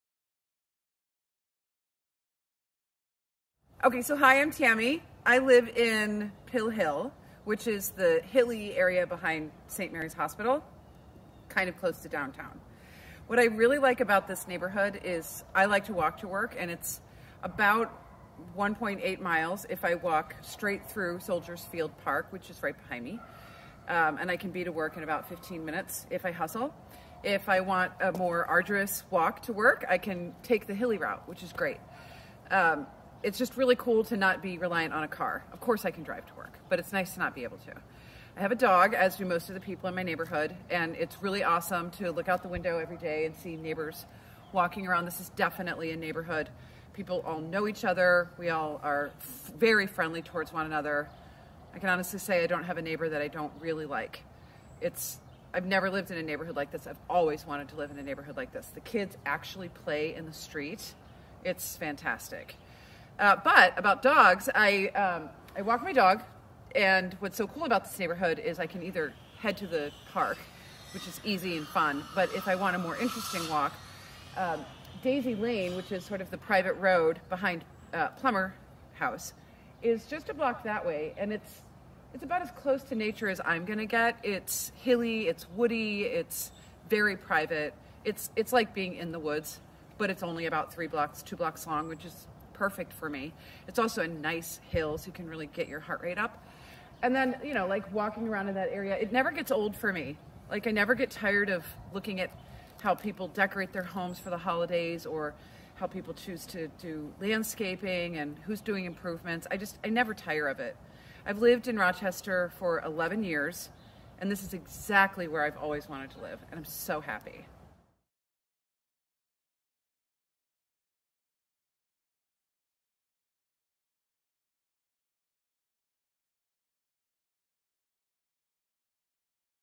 Historic Southwest Rochester MN Neighborhood Tour
Join the Realty Edge Team and Tammie Hudnall as we tour the Historic Southwest Neighborhood. Next to downtown and Soldiers Field Park the Historic Southwest places you right next to the heart of Rochester.

If you're looking to buy a house in the area or the greater Rochester area visit our website below to see listings and get in contact with an agent who will help you find your home.